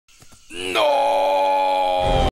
My Reaction That Humphrey (Alpha And Omega) Is Not In “Hero And Villain Swap”
Nooo, Why Is Humphrey (Alpha And Omega) Is Not In “Hero And Villain Swap”!